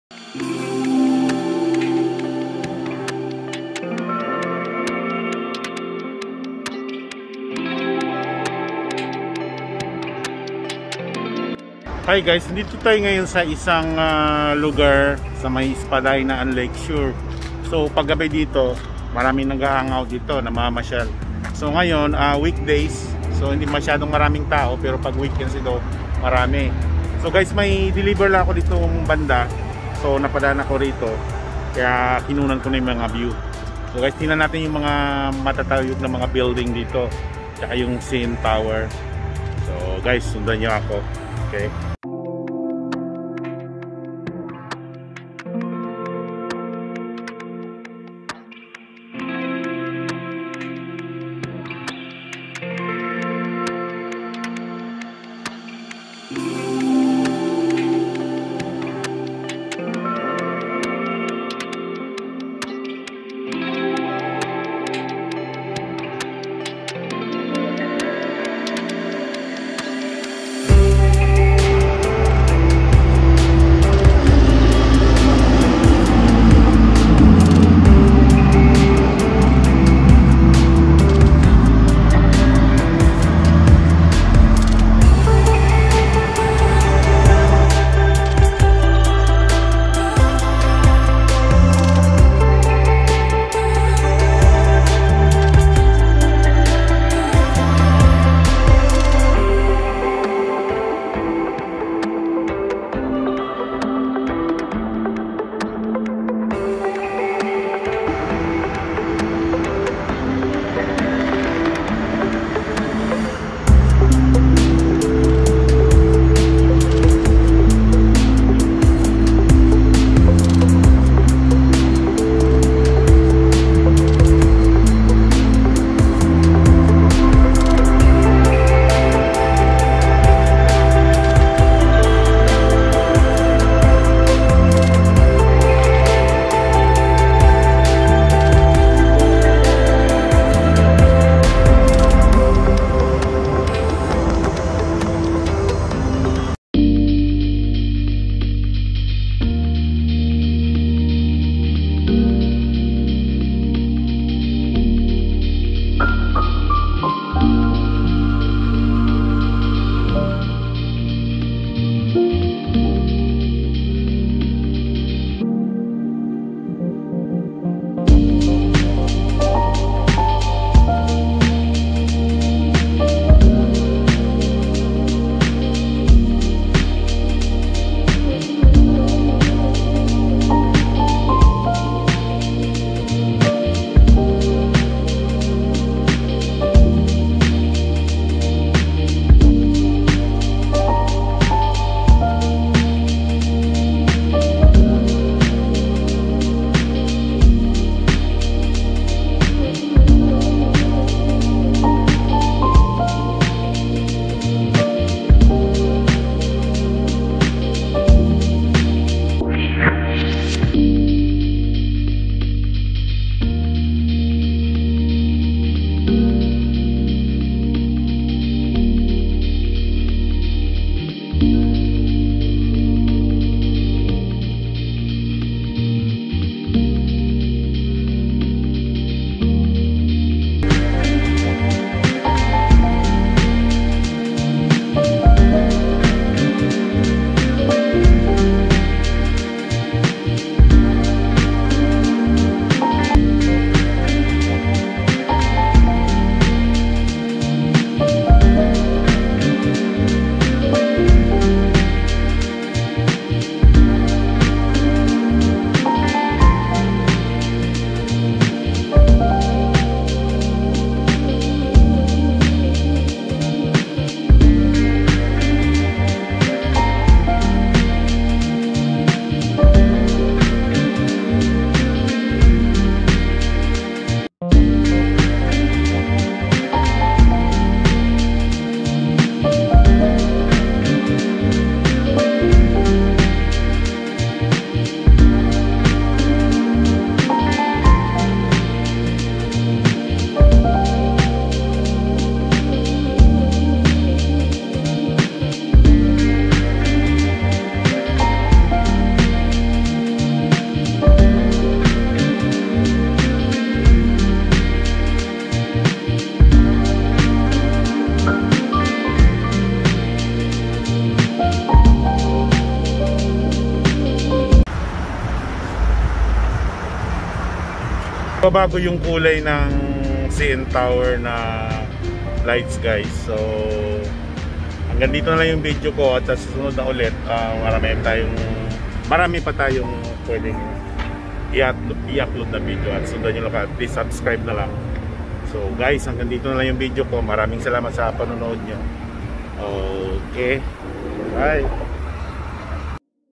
A NIGHT WALK AT TORONTO ONT CANADA
In this video i show the different places,where i was walking and delivering uber eats,So watch this video its exiting and relaxing  music..it taken at toronto downtown ontario canada